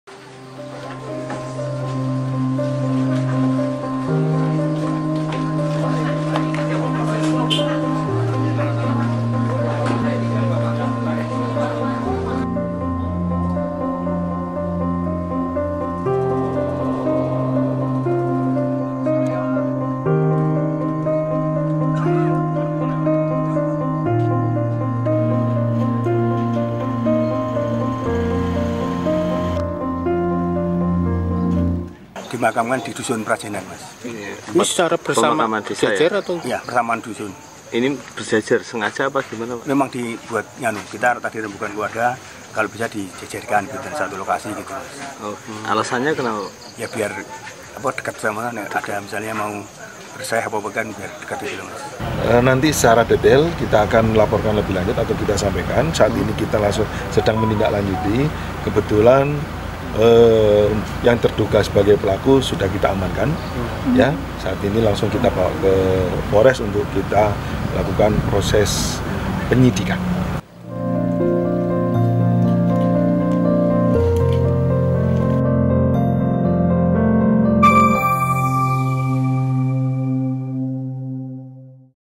Proses Pemakaman 3 Jenazah Korban Diduga Diracun di Magelang
Isak tangis mengiringi kedatangan tiga jenazah yang tewas diduga akibat diracun. Ketiganya warga Gang Durian, Dusun Prajenan, Desa Mertoyudan, Magelang. Ketiga jenazah AA (58) ayah, HR (54) ibu dan DK (25) anak kemudian disalatkan.

Setelah disalatkan, ketiganya dimakamkan di pemakaman desa. Polisi mengamankan terduga pelaku, gelas dan sendok yang digunakan korban.

Kontributor: Puji Hartono